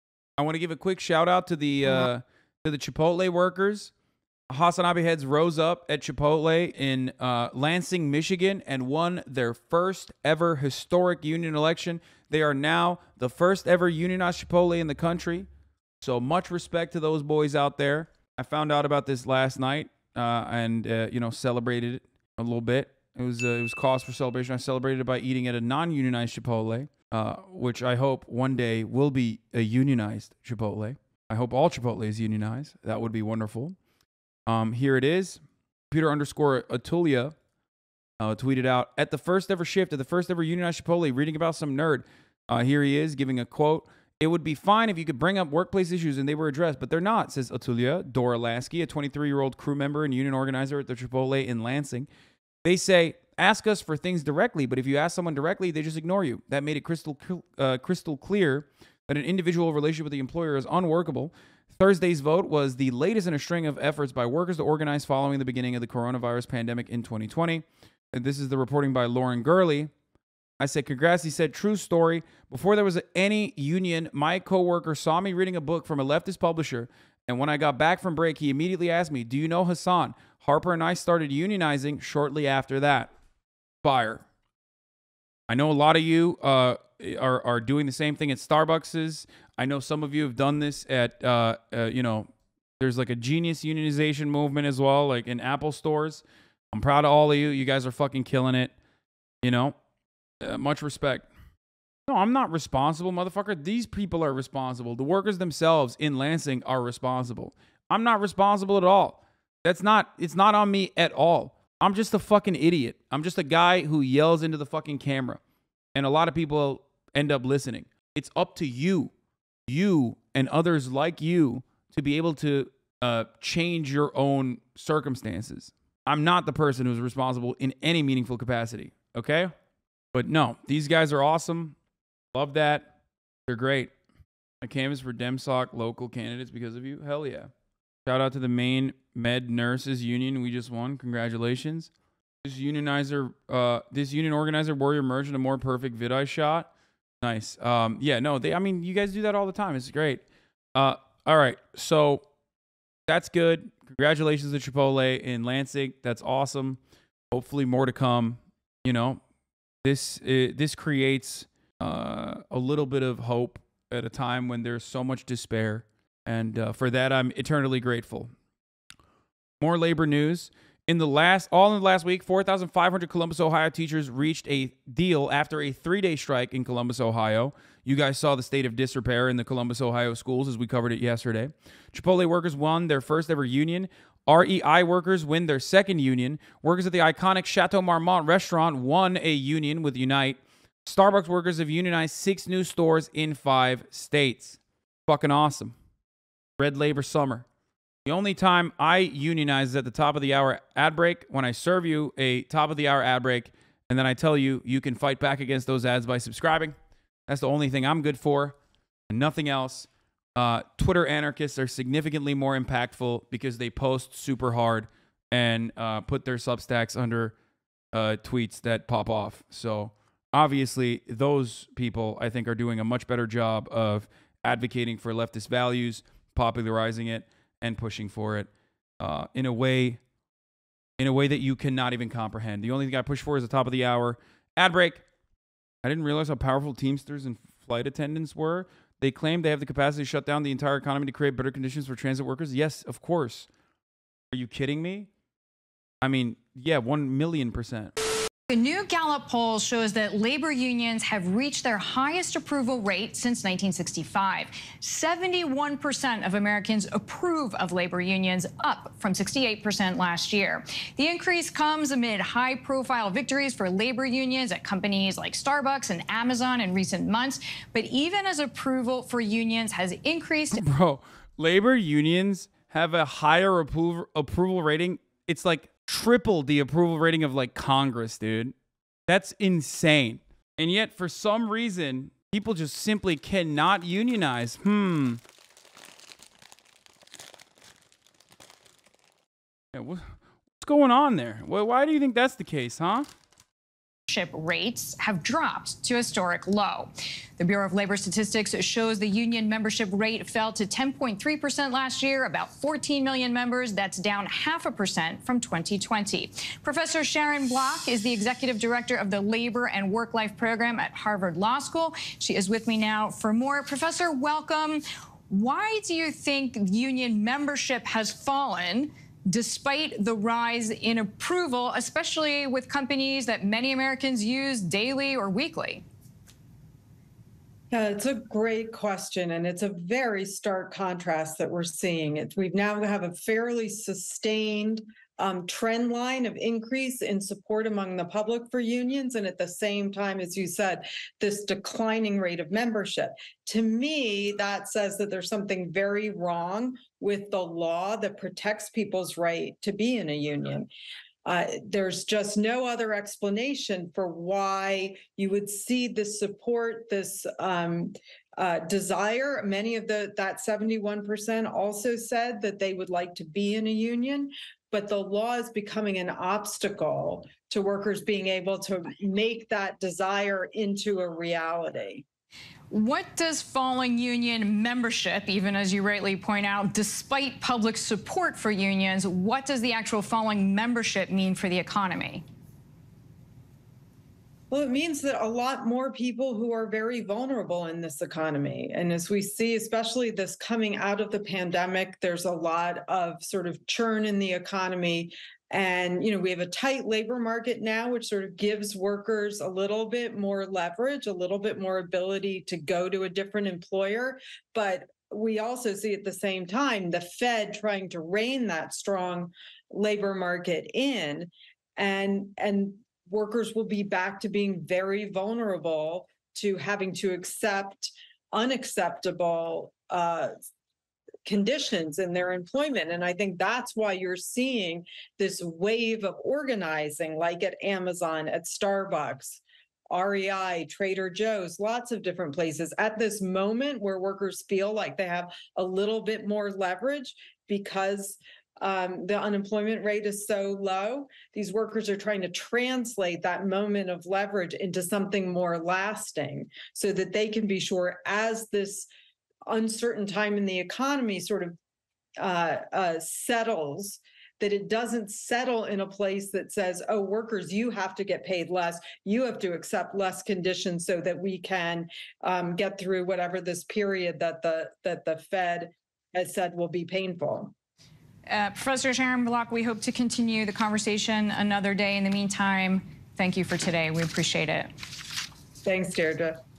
HasanAbi Heads UNIONIZED FIRST Chipotle Mexican Grill in Lansing!
Apparaently the organizers got inspired by HasanAbi's streams and took action to organize! The workers at the Lansing Chipotle voted to form a union with the International Brotherhood of Teamsters, becoming the first of the fast-casual chain's 3,000 locations to organize.

Edited by: Daily Dose of HasanAbi